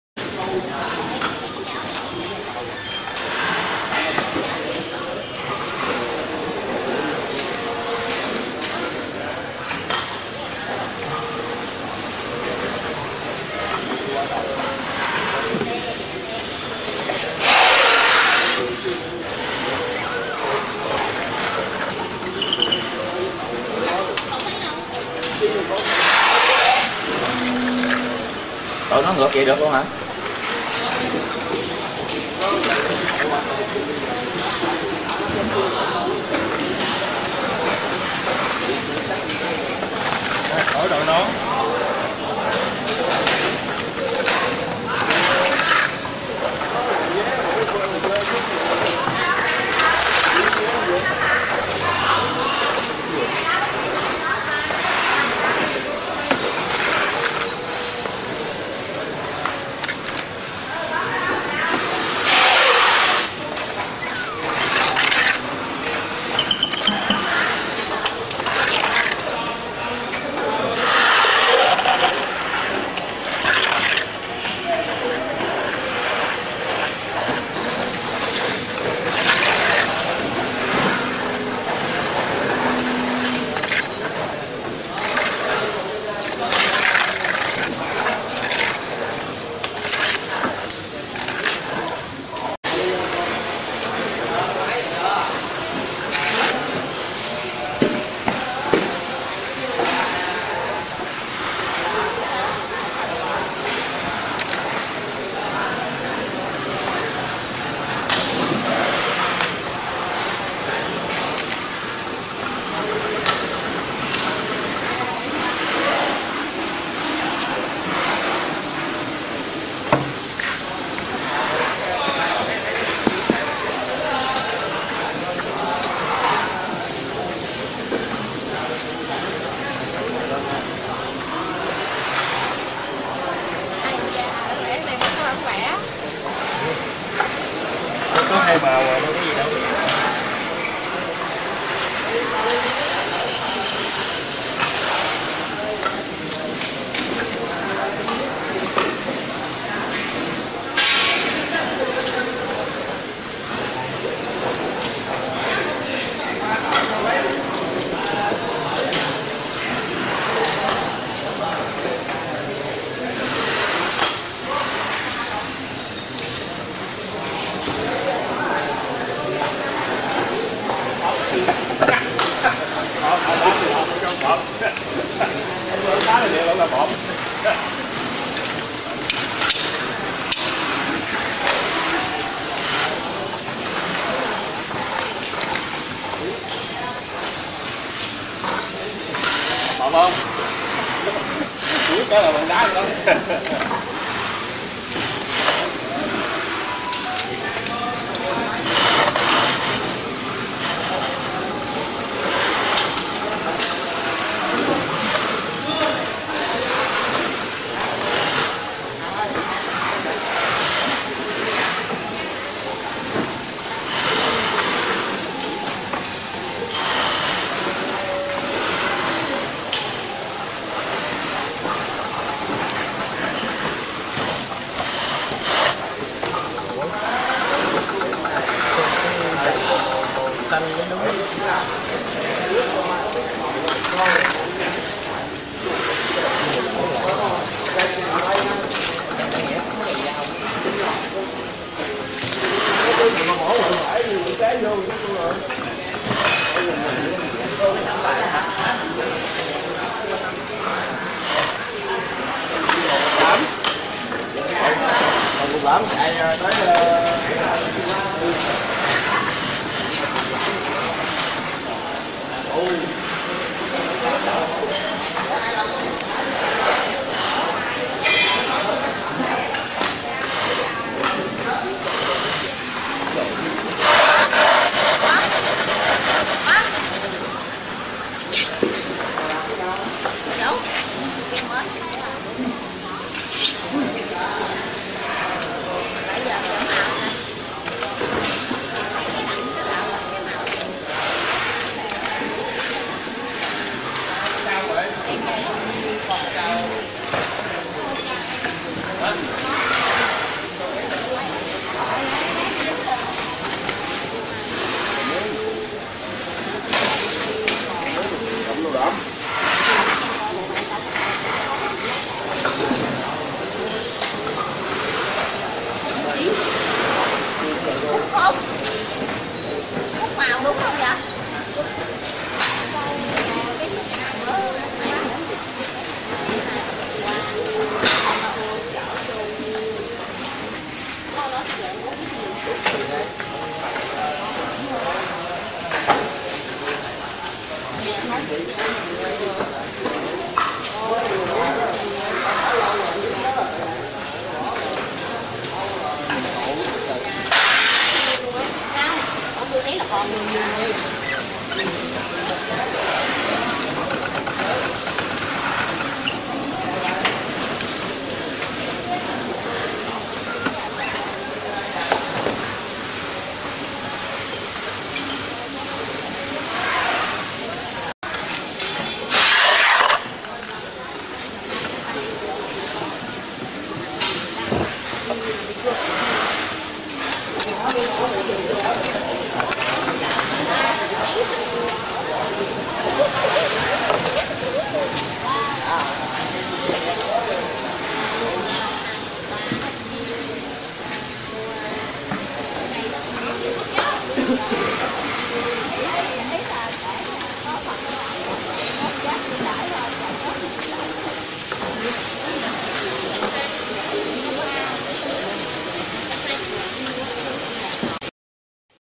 How to make cement tile
How to make an encautics tile - 100% handmade and non-burn production. Made in Vietnam - Handmade by Viettiles. What you have not known behind the clip of how the cement tile is made by handcraft-men. Premium Quality - Fast Delivery - Excellent Service
Viet Tiles Corporation
Office & Showroom: 91 Tran Nao Street, District 2, HCMC, Vietnam